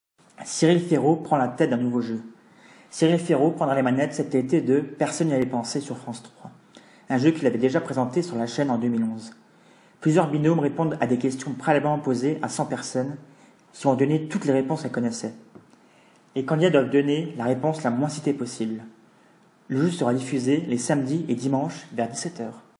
CYRIL FÉRAUD PREND LA TÊTE D’UN NOUVEAU JEU
Cyril Féraud, prendra les manettes cet été de « Personne n’y avait pensé », sur France 3, un jeu qu’il avait déjà présenté sur la chaîne en 2011.